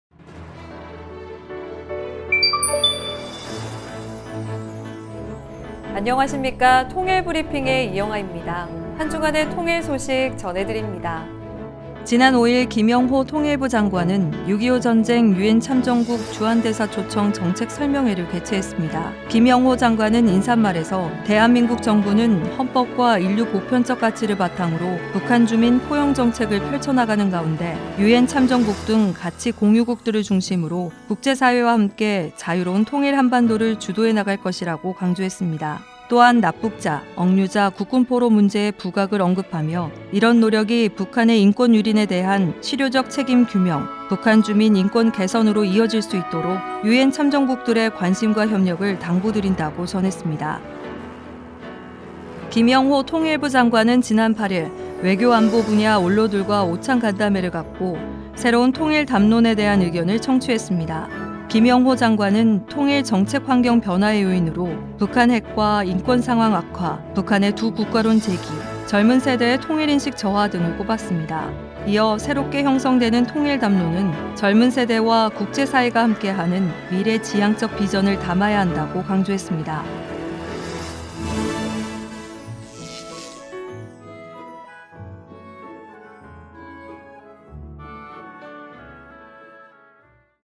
[통일브리핑] 6.25 전쟁 유엔참전국 주한대사 초청 정책설명회(4월 셋째 주)
통일브리핑
  - 6.25 전쟁 유엔참전국 주한대사 초청 정책설명회
  - 외교•안보 분야 원로 오찬 간담회